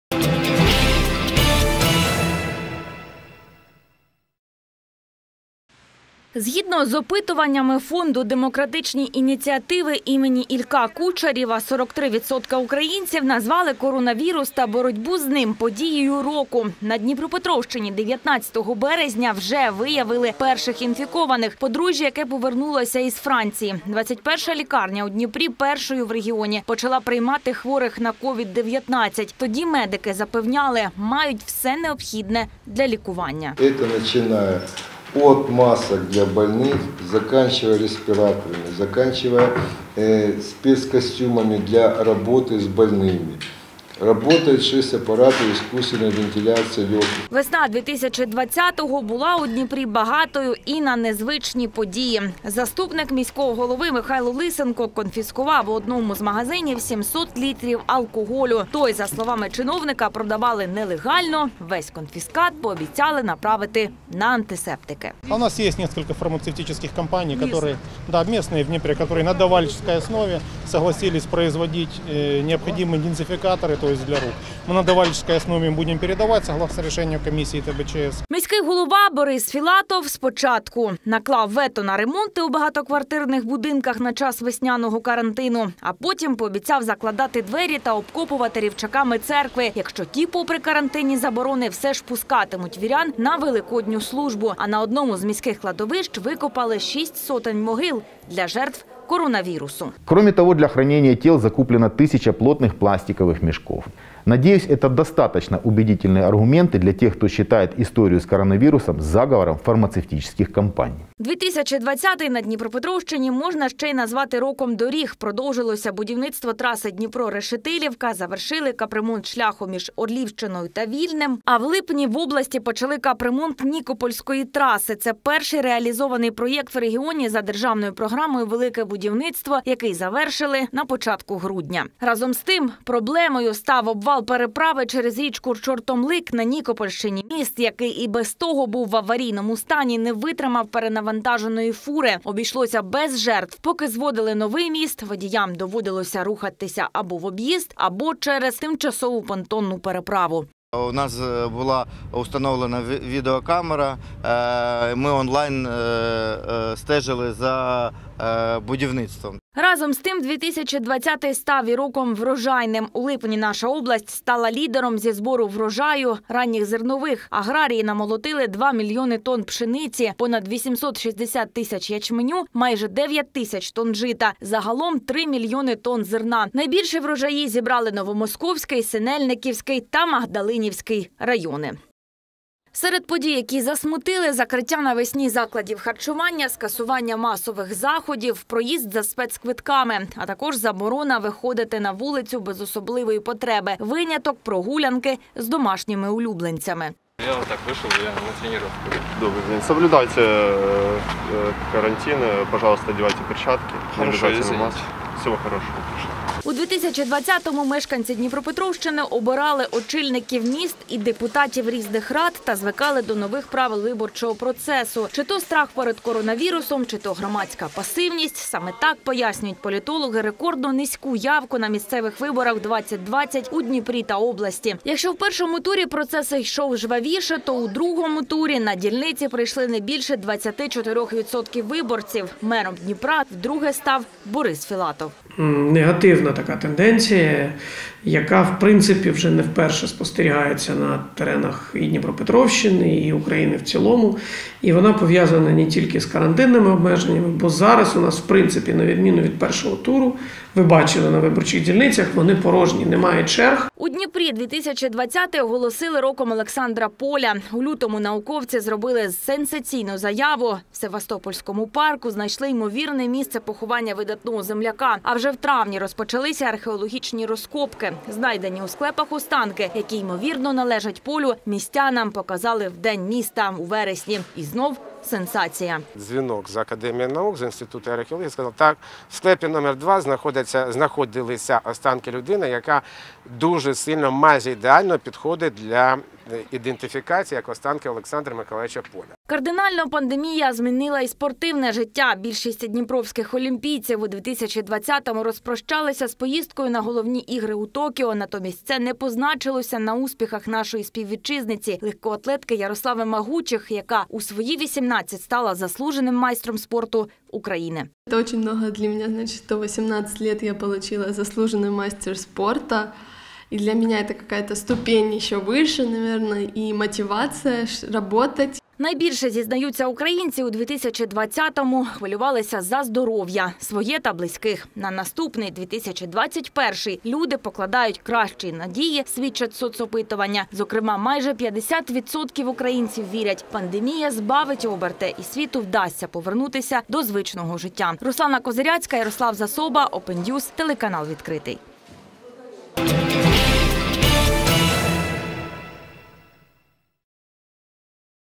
Не коронавірусом єдиним: чим запам'ятався 2020-й рік у Дніпрі та області?
Журнал «Тайм» назвав 2020-й найгіршим роком в історії. А українці в соціологічних опитуваннях охрестили його «роком поганих подій». Майже половина наших співвітчизників назвали подією року епідемію КОВІД-19.  Разом з тим, у вирі цих подій є й приємні новини, спогади та факти.  Чим мешканцям Дніпропетровщини запам’ятався рік, що минає, зібрали у дайджесті журналісти "Відкритого". _______________________________________________